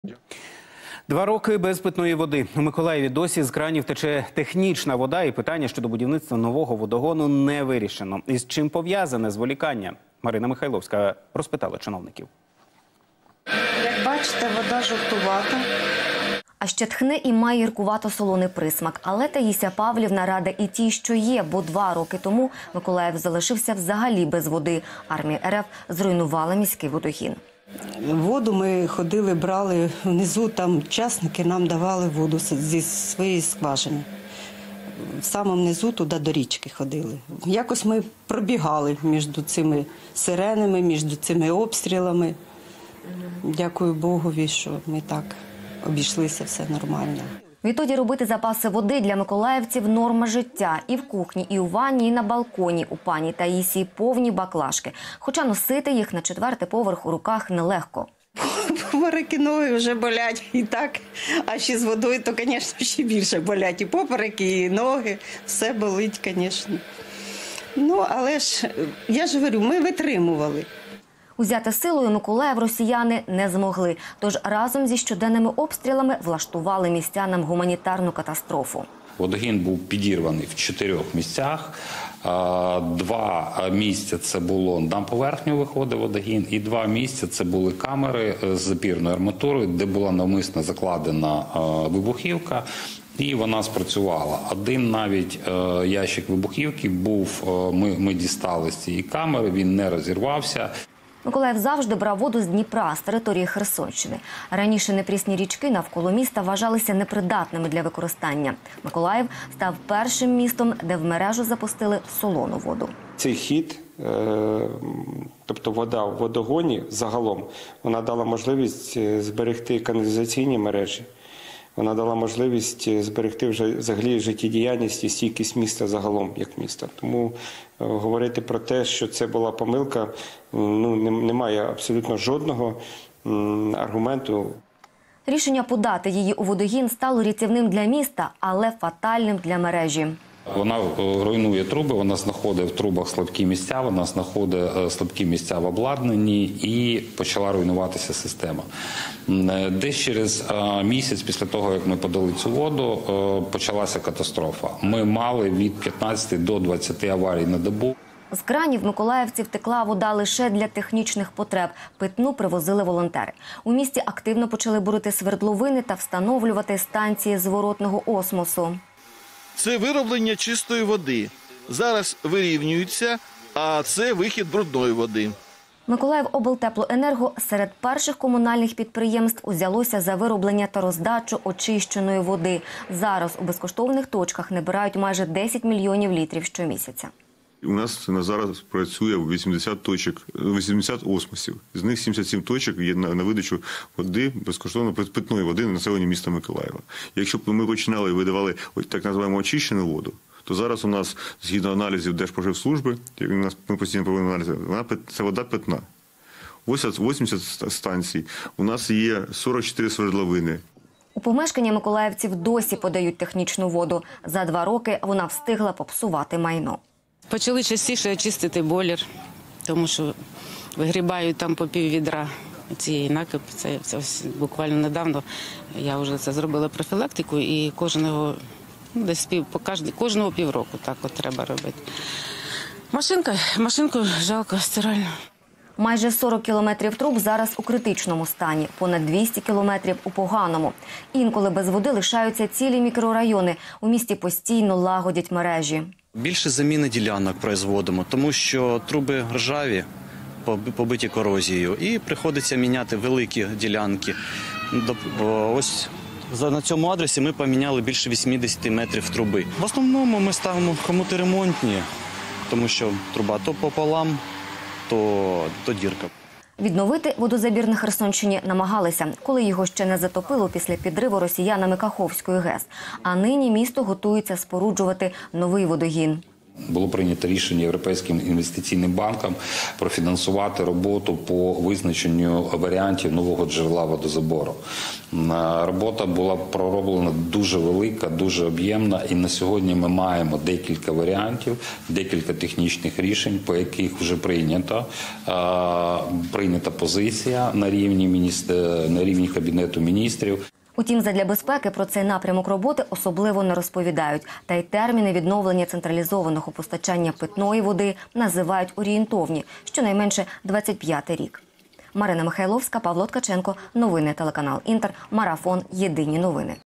Два роки без води: як виживають люди у Миколаєві
Два роки без питної води. У Миколаєві досі з кранів тече технічна вода і питання щодо будівництва нового водогону - не вирішено. І з чим пов'язане зволікання? Марина Михайловська питала чиновників.